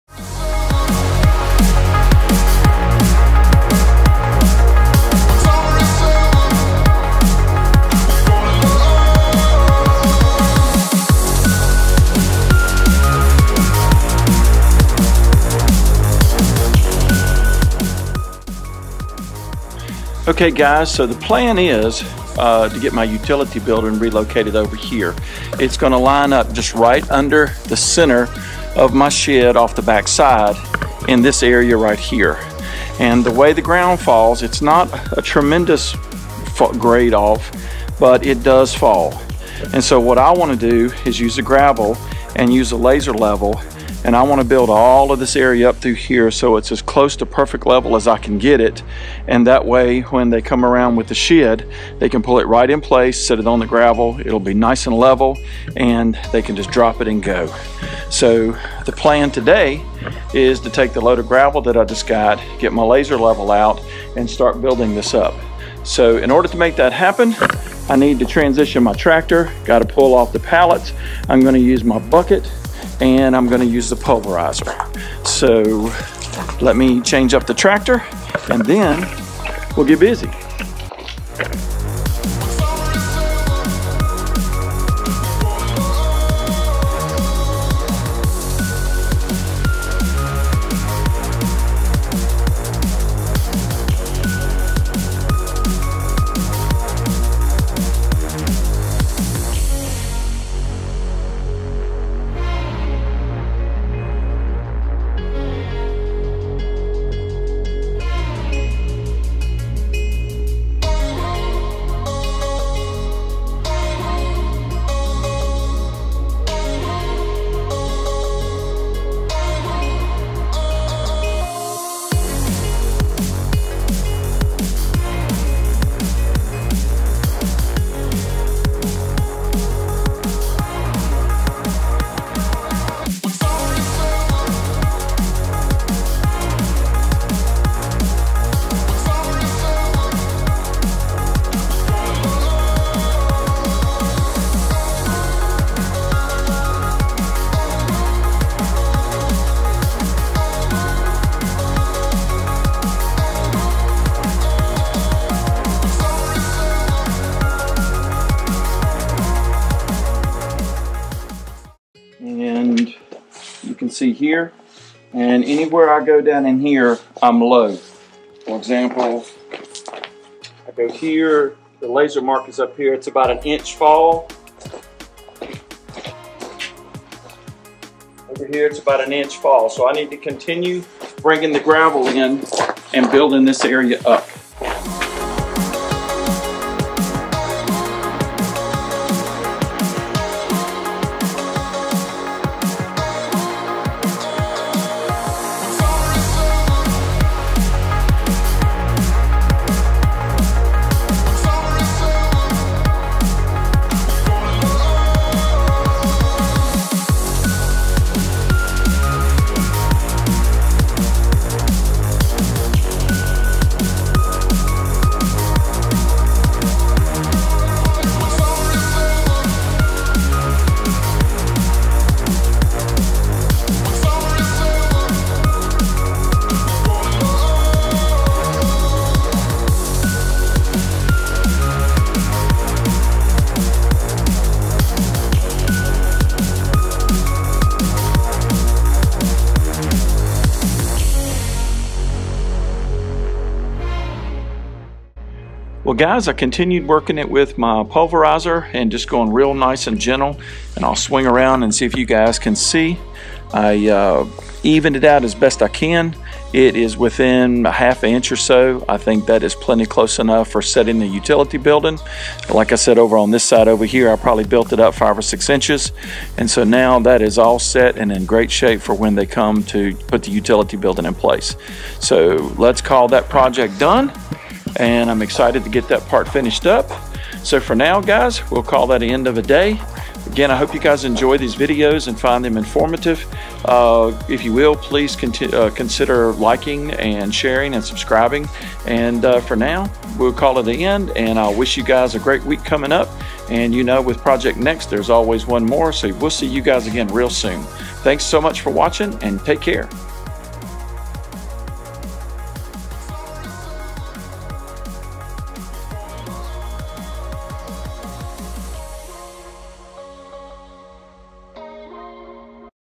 JOHN DEERE TRACTORS / SPREAD & LEVEL GRAVEL
JOHN DEERE TRACTORS / SPREADING GRAVEL / Spread and level gravel using a JD 3032 Pulverizer and a laser level. Amazing Results !